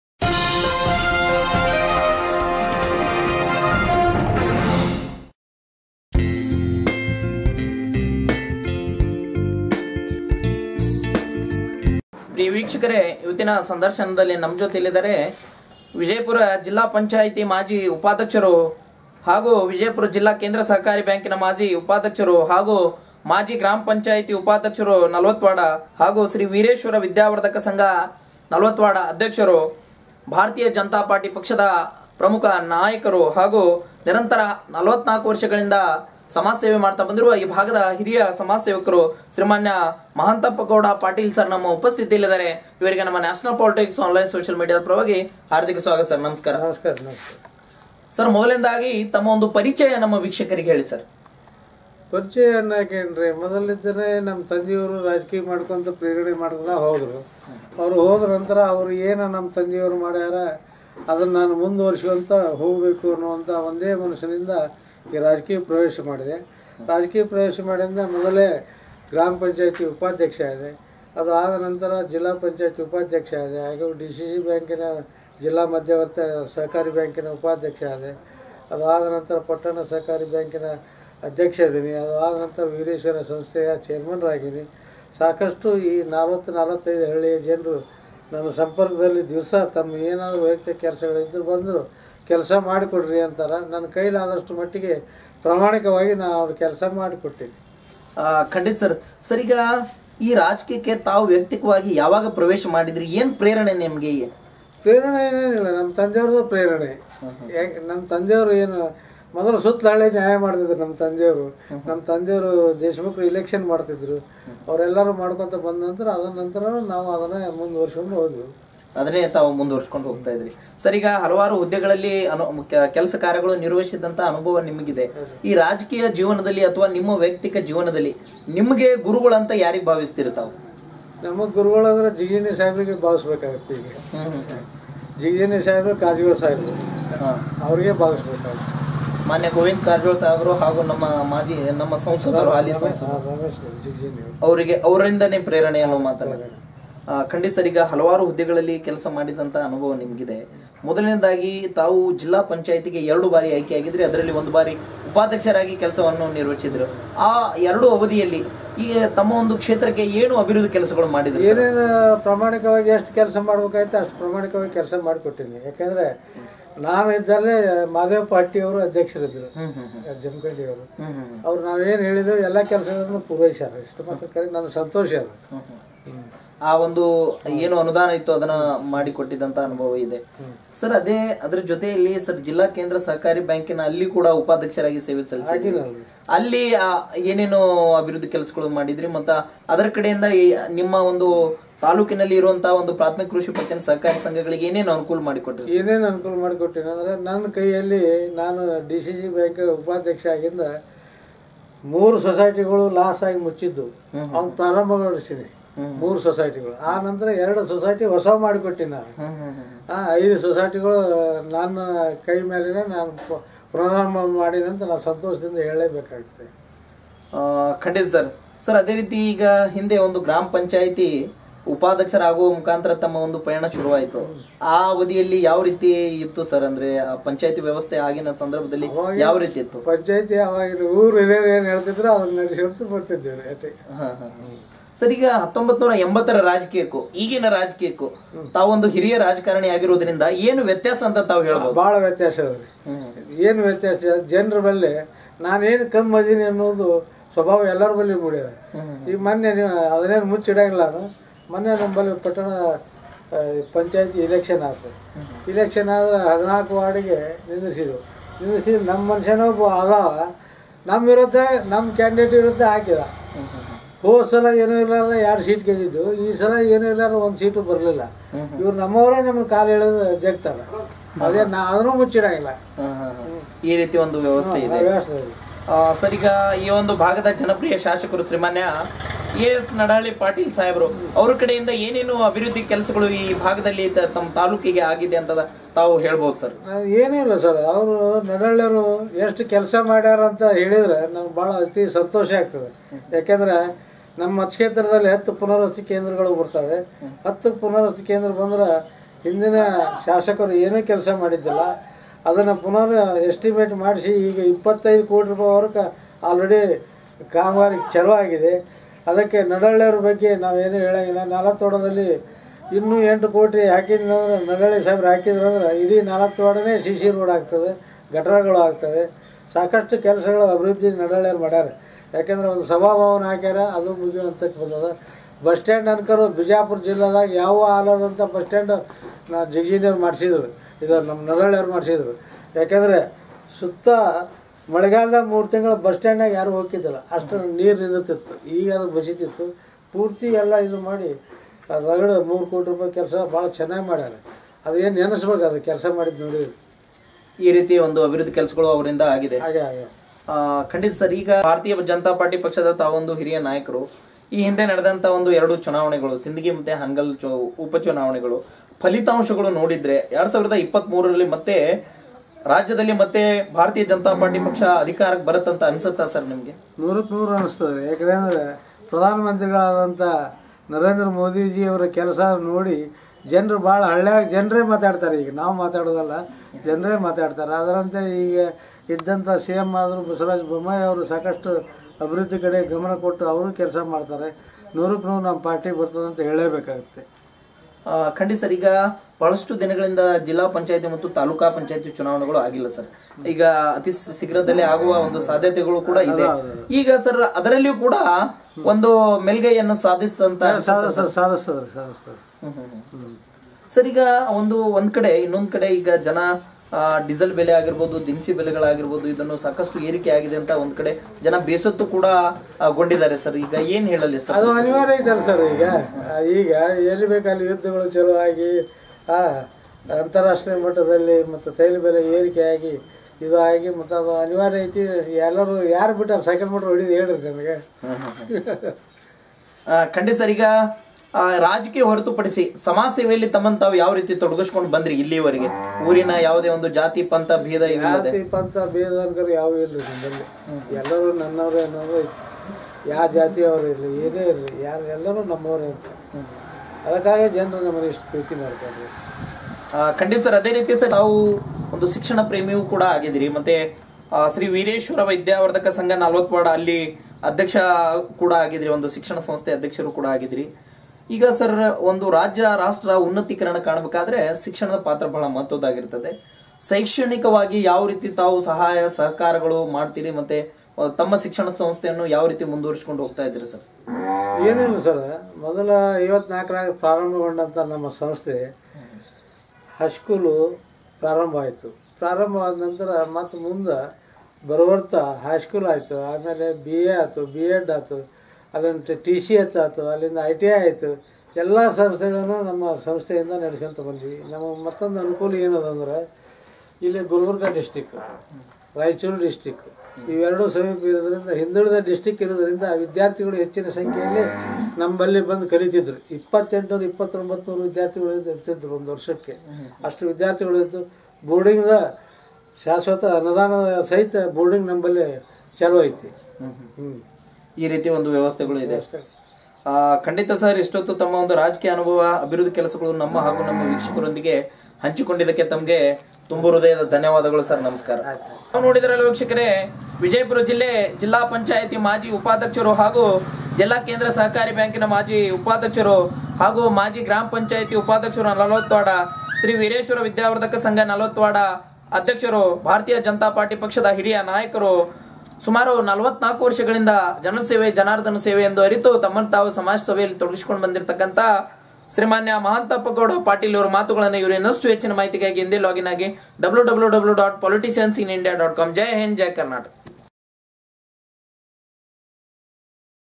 M S Patil [ ಮಾಜಿ ಉಪಾಧ್ಯಕ್ಷರು ಜಿಲ್ಲಾ ಪಂಚಾಯತ್ : ವಿಜಯಾಪುರ ]
ಮಾಜಿ ಉಪಾಧ್ಯಕ್ಷರು ವಿಜಯಪುರ ಜಿಲ್ಲಾ ಕೇಂದ್ರ ಸಹಕಾರ ಬ್ಯಾಂಕ್, ವಿಜಯಪುರ.
ಮಾಜಿ ಜಿಲ್ಲಾ ಪಂಚಾಯತ್ ಸದಸ್ಯರು ವಿಜಯಾಪುರ (ಎರಡು ಅವಧಿಗಳು).
ಅಧ್ಯಕ್ಷರು ಶ್ರೀ ವೀರೇಶ್ವರ ವಿದ್ಯಾವರ್ಧಕ ಸಂಘ ನಾಲತವಾಡ .
ಮಾಜಿ ಉಪಾಧ್ಯಕ್ಷರು ಗ್ರಾಮ ಪಂಚಾಯತ್ : ನಾಲತವಾಡ .
ಭಾರತೀಯ ಜನತಾ ಪಾರ್ಟಿ (ಬಿಜೆಪಿ) ಮುಖಂಡರು .
ಮತ್ತು ಸಮಾಜ ಸೇವಕರು ( 48 ವರ್ಷಗಳಿಂದ).